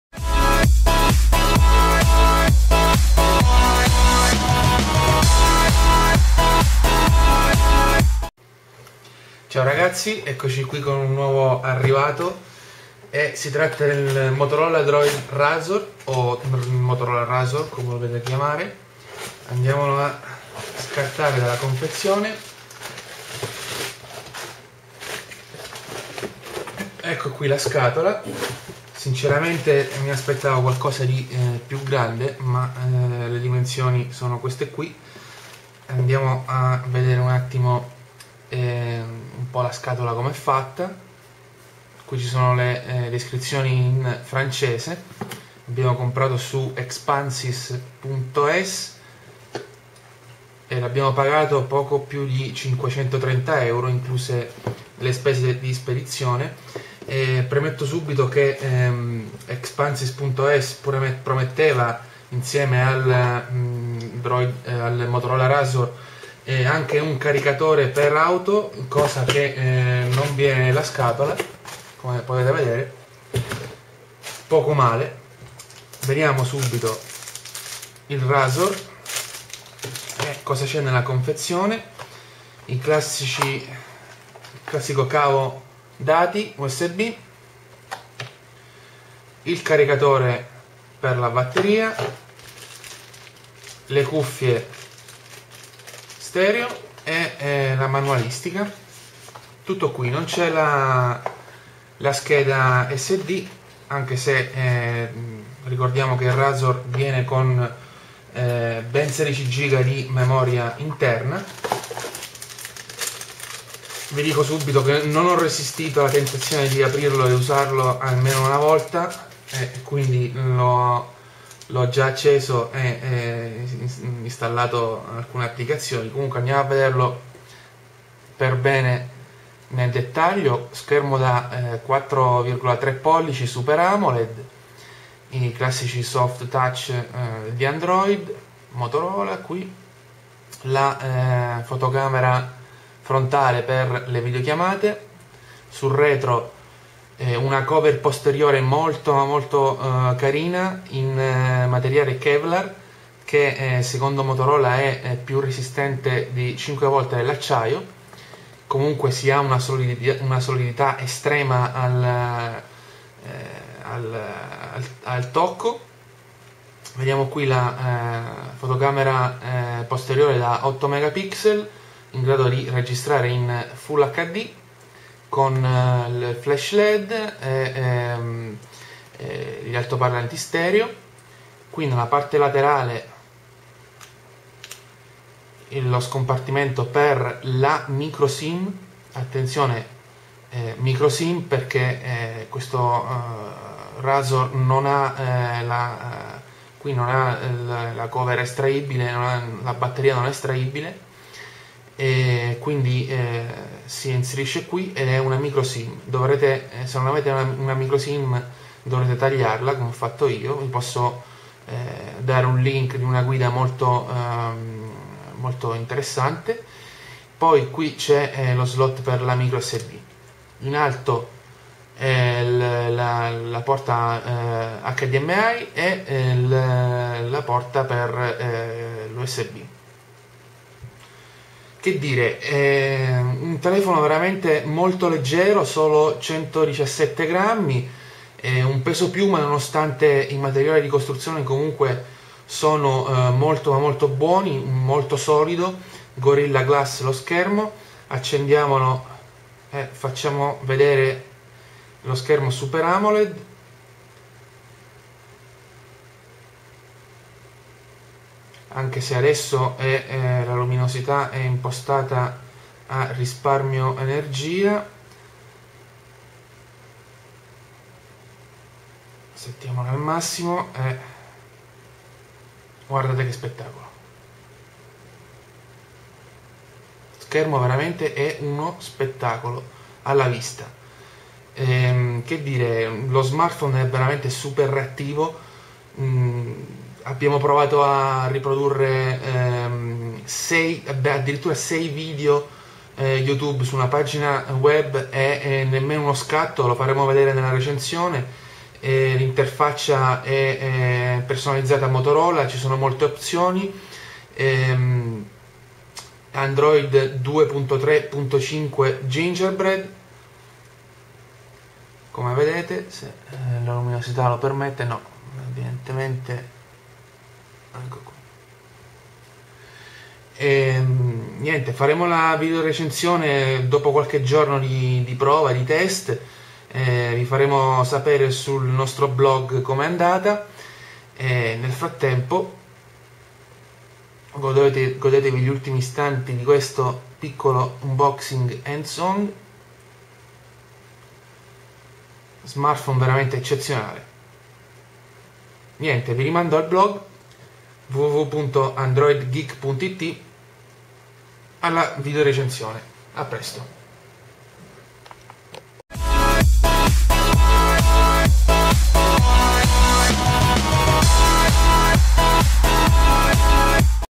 Motorola Razr, unboxing e prime impressioni
È arrivato il nostro nuovo giocattolino, si tratta del nuovissimoMotorola Razr. In attesa di una video recensione cometa, vi proponiamo un video unboxing + prime impressioni del nuovo top gamma della casa alata. X saperne di più